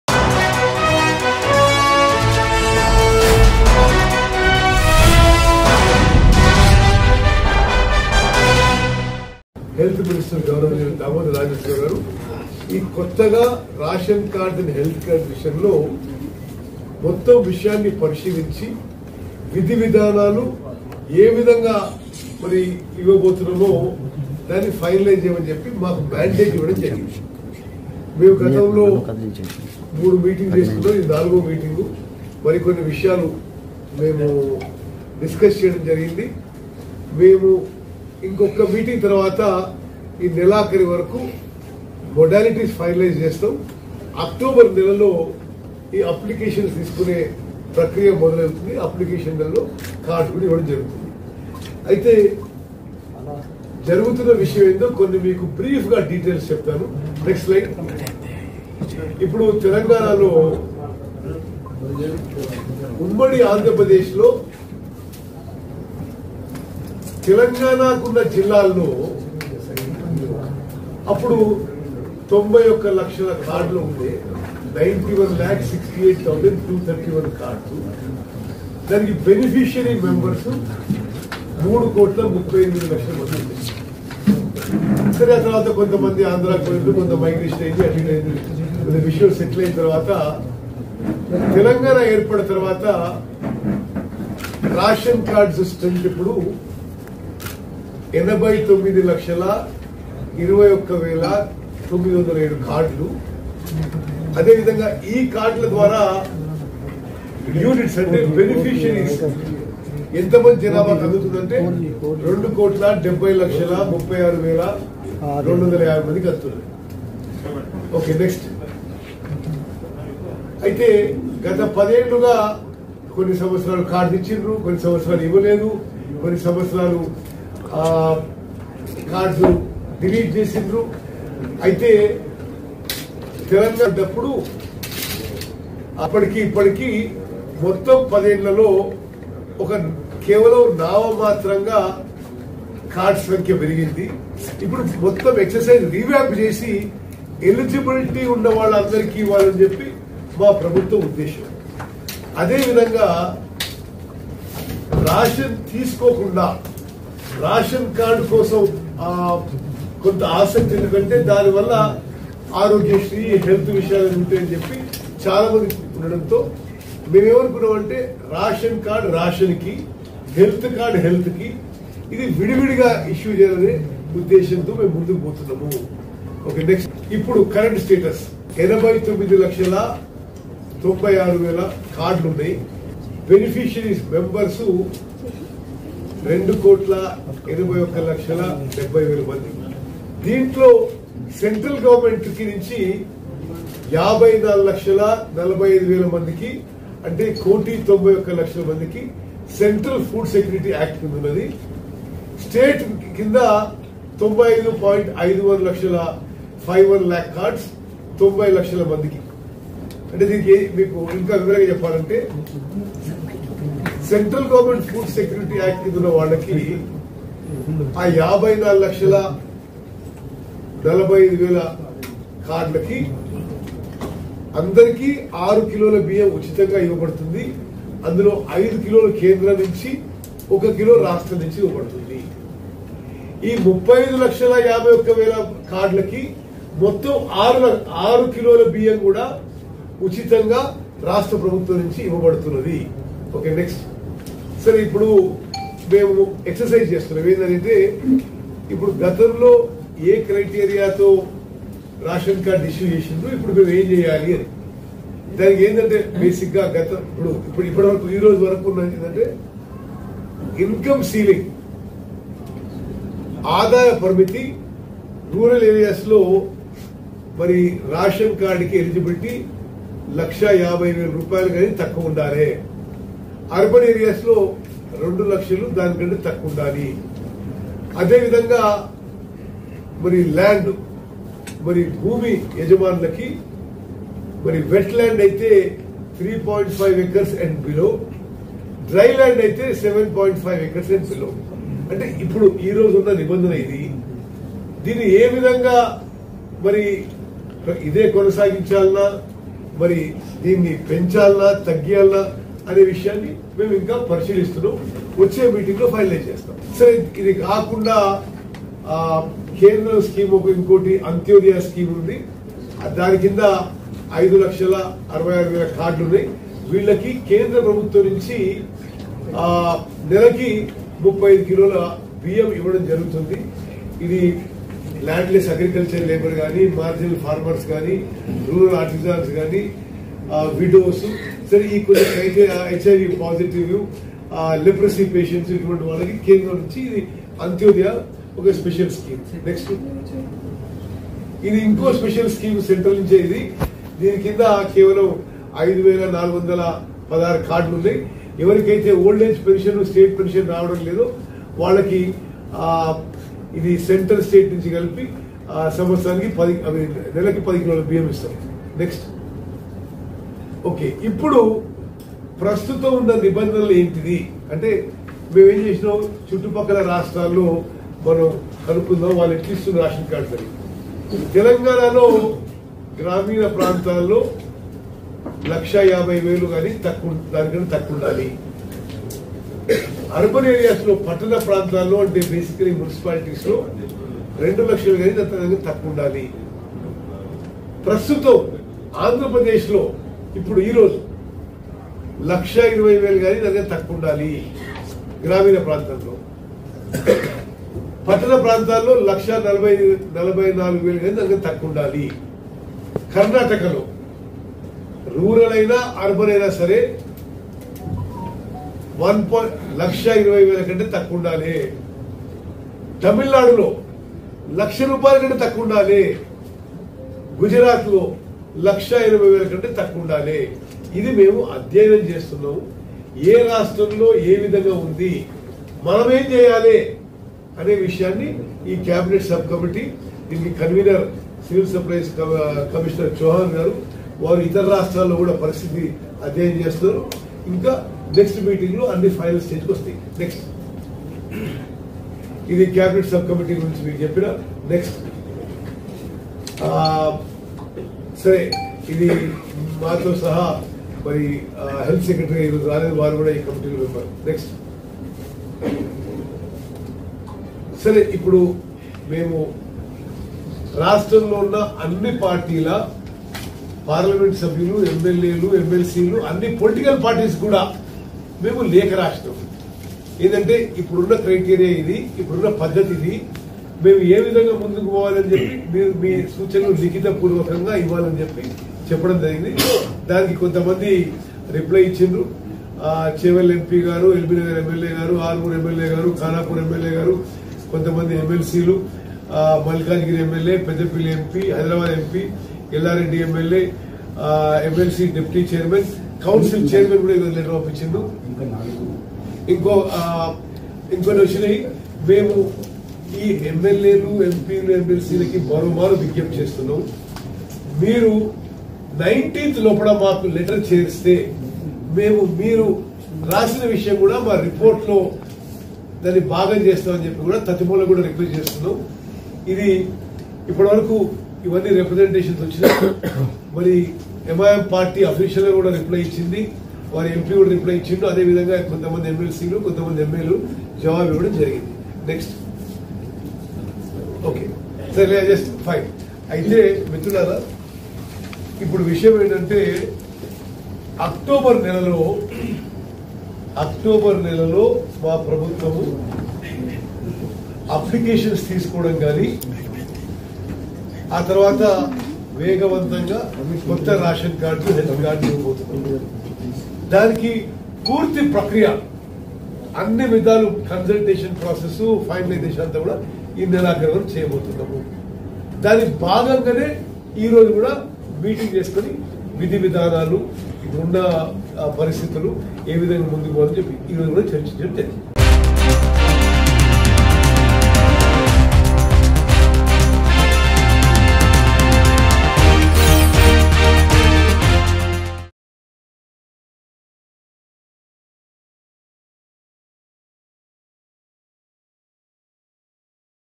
Minister Uttam Kumar Reddy Announcement On New Ration Cards | Congress | Telangana | Mango News
Minister Uttam Kumar Reddy Announcement On New Ration Cards. He said that after the final meeting of the subcommittee, the government will accept new ration card applications, and the cards will be used starting in October.